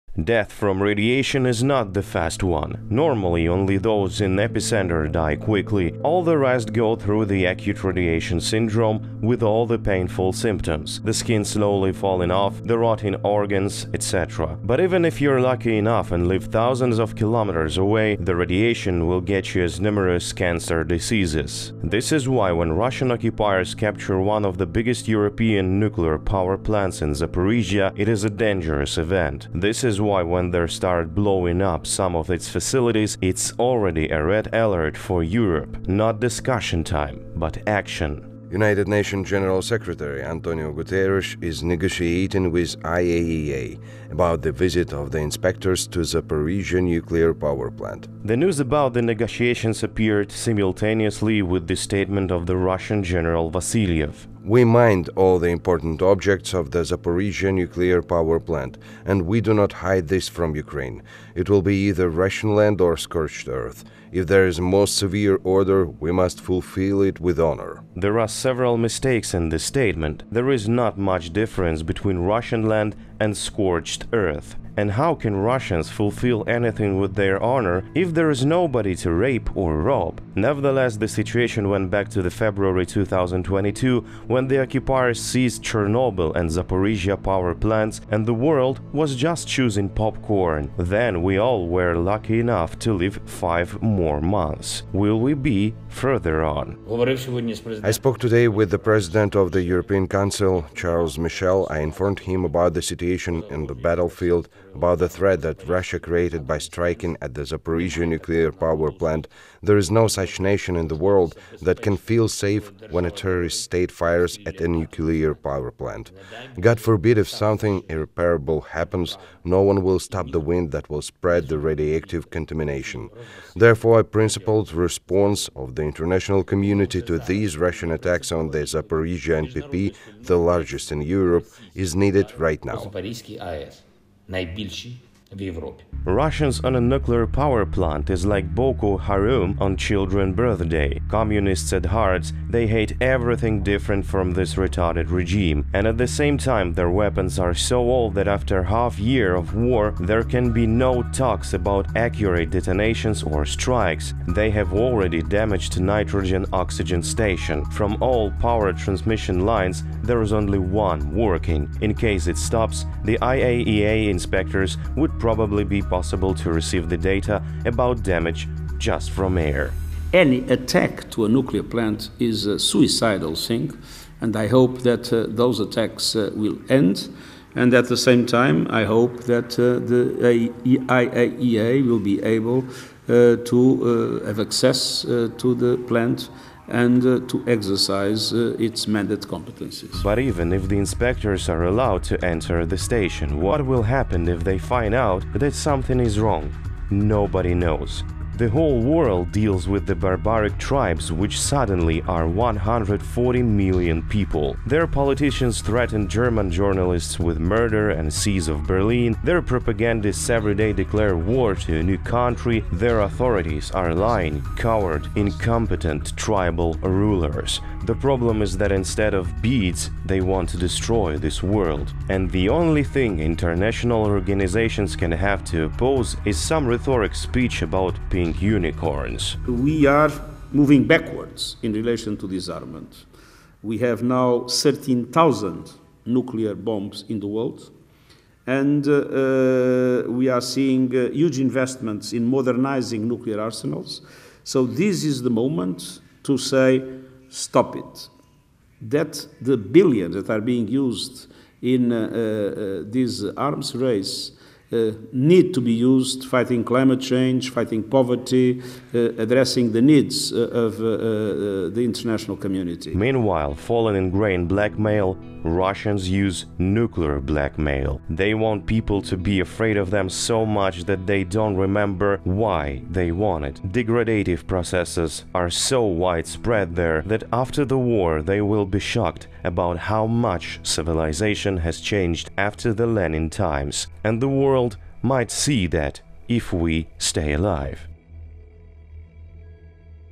Nuclear blackmail. russian occupiers mined Zaporizhzhya Power Plant and wait for "severe order"
Nuclear terrorism in the heart of Europe.
russian occupiers declare that they have mined Zaporizhzhya NPP. The biggest NPP in Europe.
IAEA and UN slowly begin to negotiate, who's going to come and check.
Nothing urgent.
It's just a nuclear power plant.